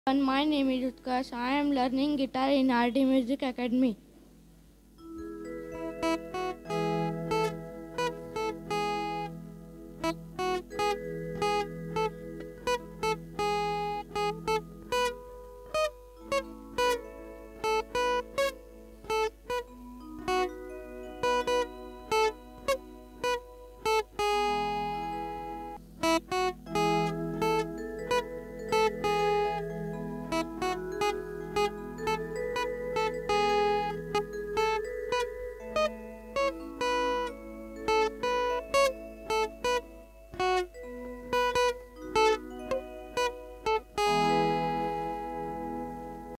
Participant Utkarsh | Student | RD Music Academy | Rohit Duggal
To Join Online and Offline Music Classes Contact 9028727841/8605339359 !
Utkarsh Thorat is Student of RD Music Academy learning Guitar ! He is from Shrirampur and participated in online competition organised by RD Music Academy! Give Maximum Likes to make her win and Remember To Share himVideo! To Join Offline and Online Classes Contact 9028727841/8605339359 !